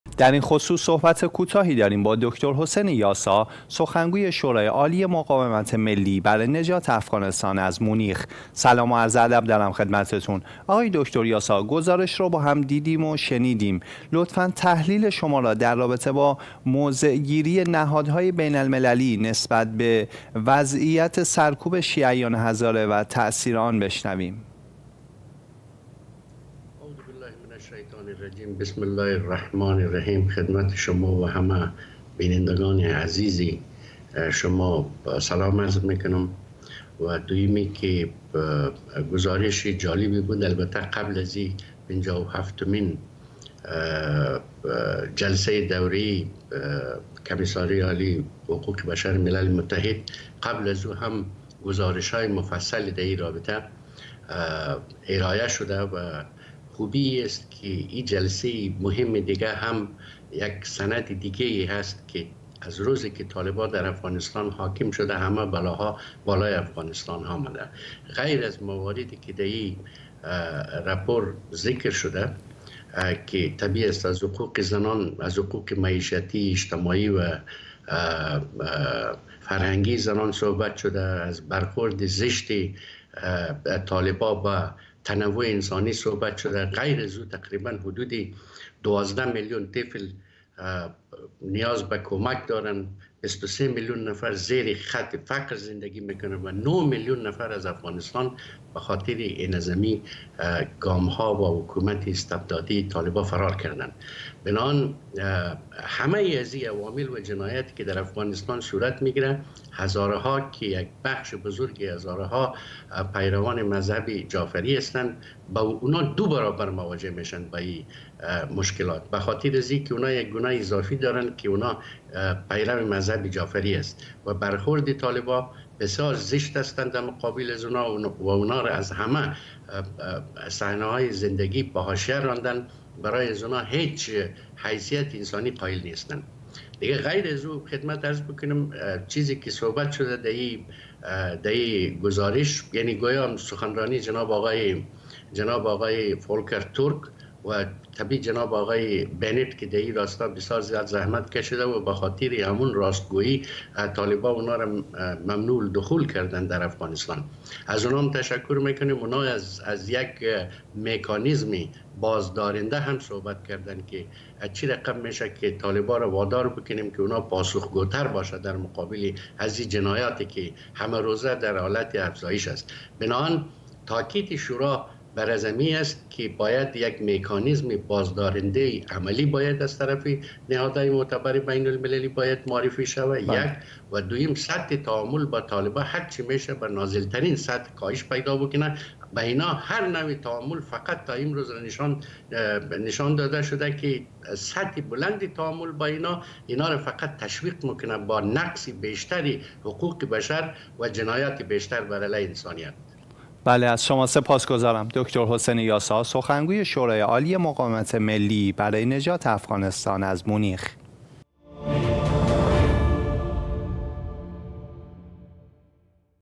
گفتگو با دکتر حسین یاسا در مورد سرکوب شیعیان هزاره توسط طالبان
سرکوب شیعیان هزاره توسط طالبان همچنان یکی از بحران‌های بزرگ انسانی در افغانستان است. در این گفتگو، دکتر حسین یاسا به بررسی وضعیت جاری و تاثیرات این سرکوب بر جامعه هزاره‌ها می‌پردازد.

 🔔 همین حالا مشترک شوید و از تحلیل‌های بیشتر باخبر شوید!

 00:00 - مقدمه و معرفی دکتر حسین یاسا
00:30 - وضعیت شیعیان هزاره پس از روی کار آمدن طالبان
01:15 - بررسی فشارهای سیاسی و اجتماعی بر جامعه هزاره‌ها
02:45 - واکنش جامعه جهانی به سرکوب هزاره‌ها
03:10 - راه‌حل‌ها و پیشنهادات برای حمایت از شیعیان هزاره